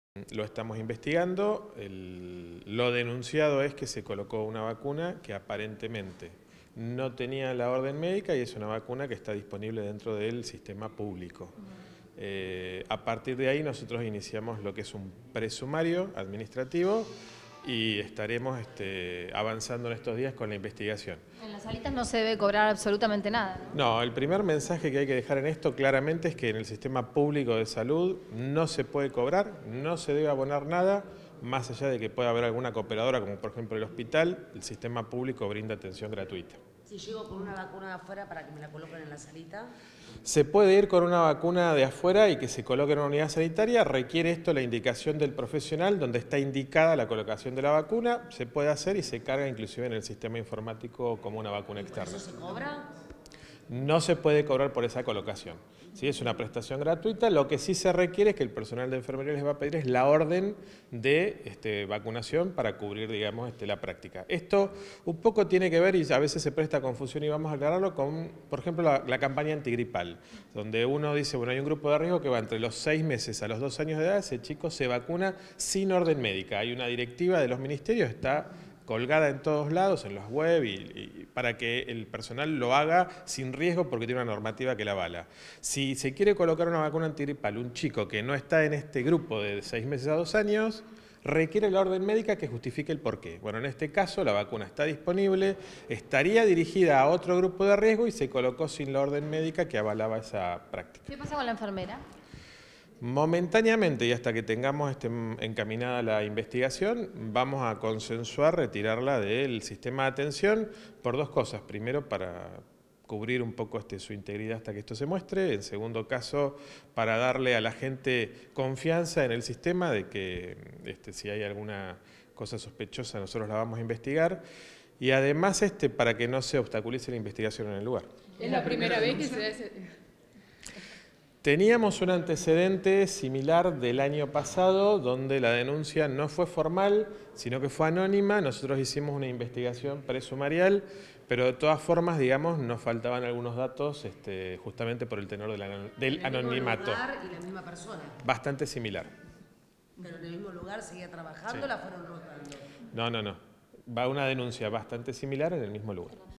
GRAVE DENUNCIA
Apartarán a la enfermera que cobraba vacunas en la salita del barrio Napostá.